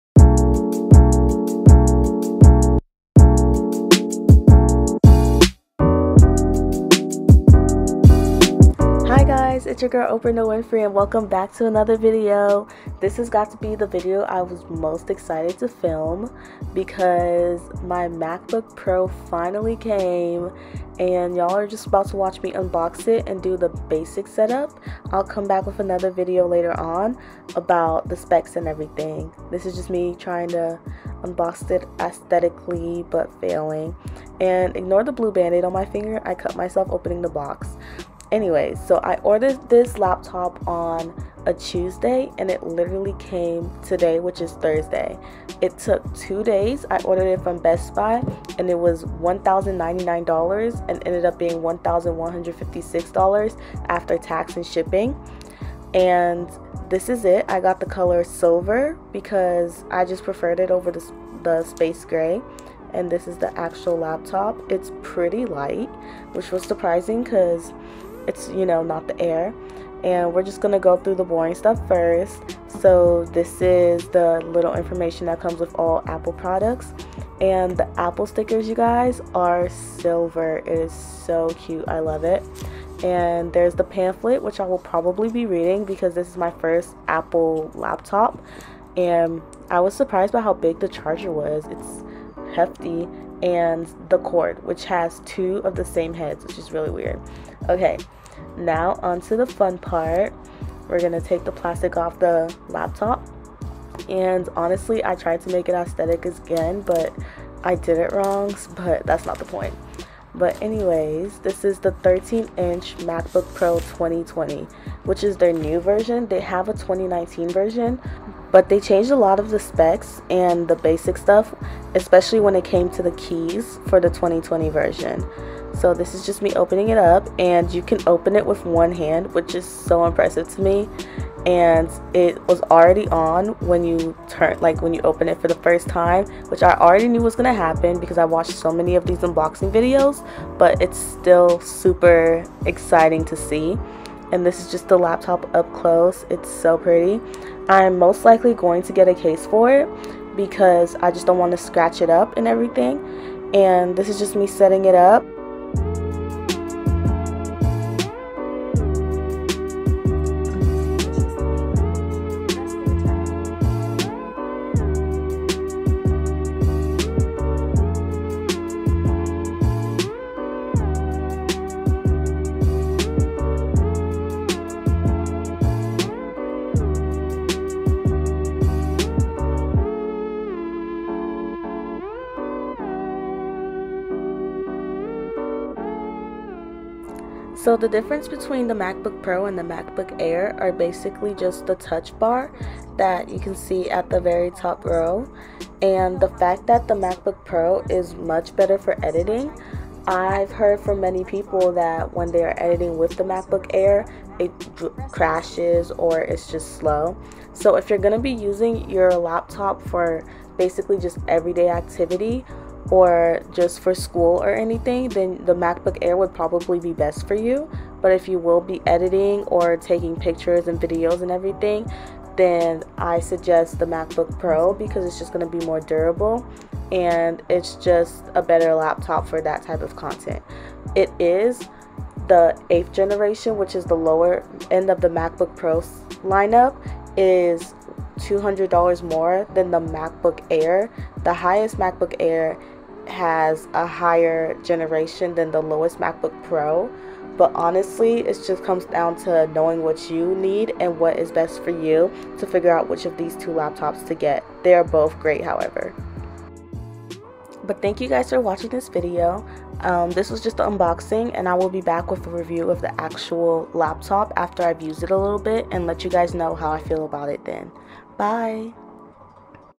2020 13 IN MACBOOK PRO UNBOXING AND BASIC SETUP || SILVER
I was able to finally buy myself the 2020 macbook pro and cannot wait to show you guys the specs! Review coming soon.

~Support the Black Lives Matter Movement~

Sign-